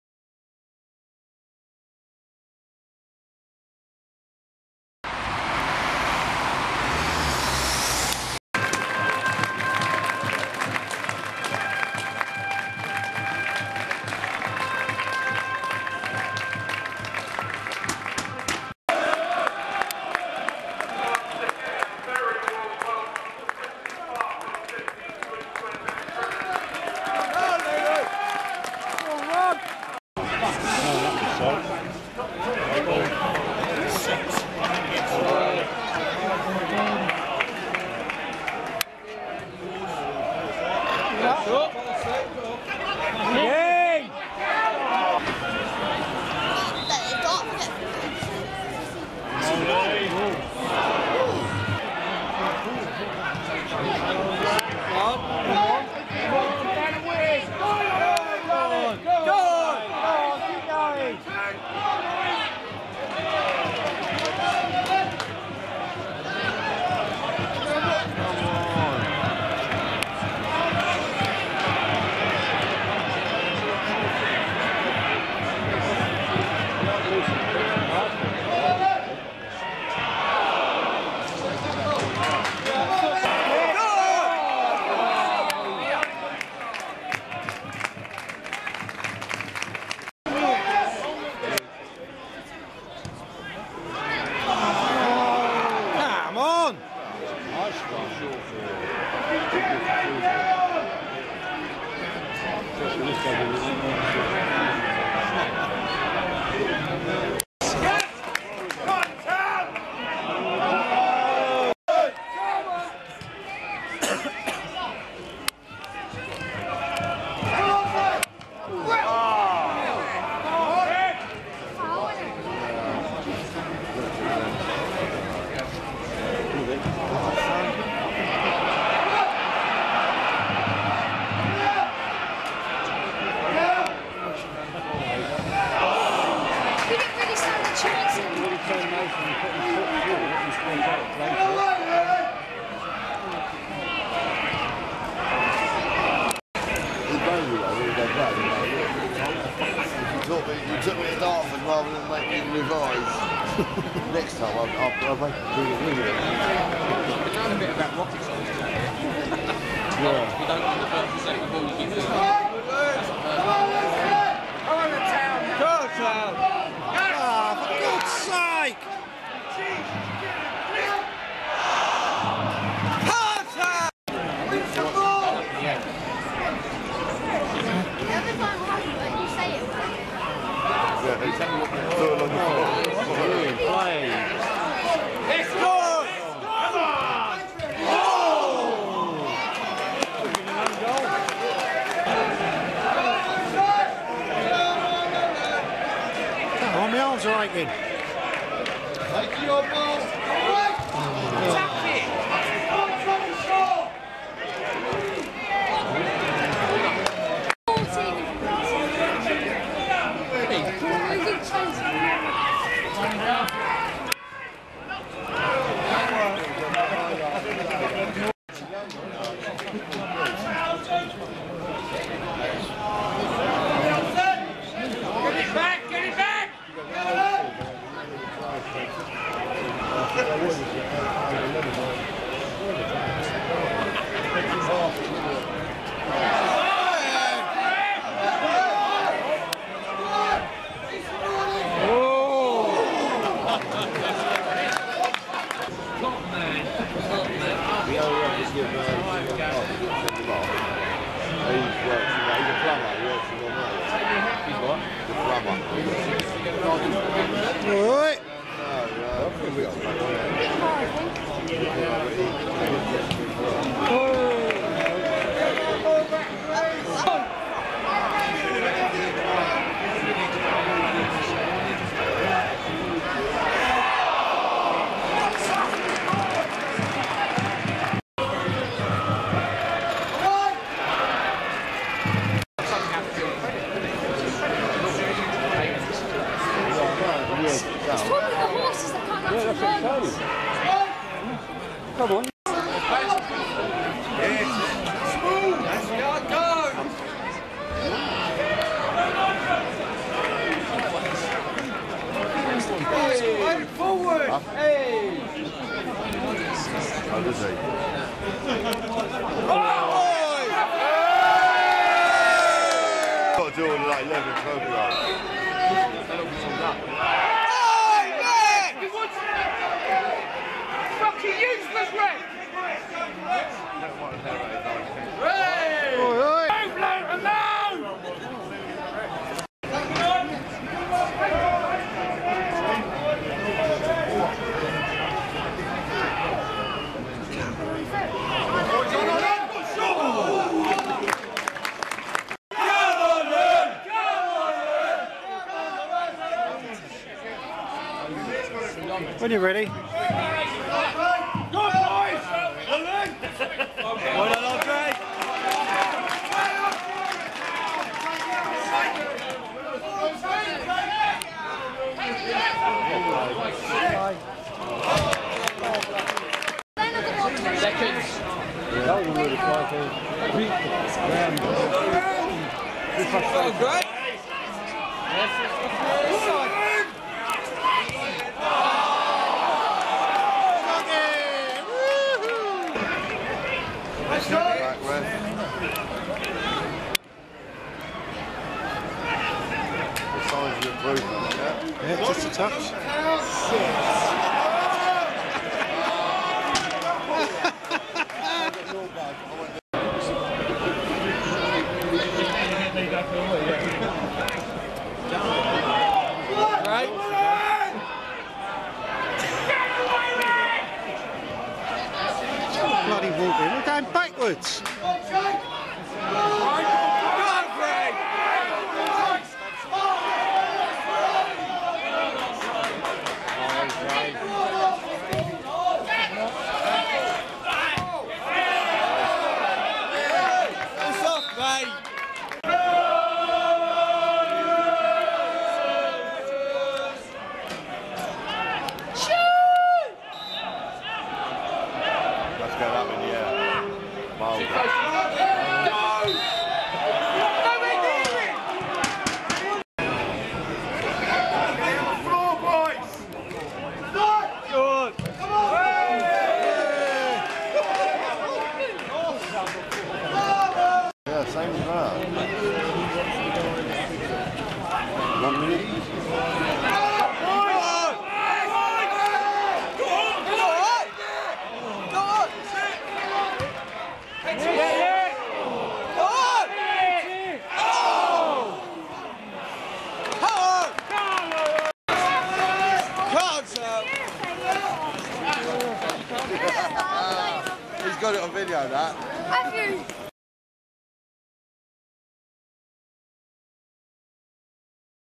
Dartford FC 1-0 Luton Town Lge 1st Half Incidental Clips 12th Feb 2013
Luton go down to their tenth defeat of an already hugely disappointing season (Bar the amazing cup run)Growing concern amongst the fans was once again highlighted this time at Princes Park where a spectacular goal from Ryan Hayes settled the game.Oh well It`s COYH Time against the Lions next.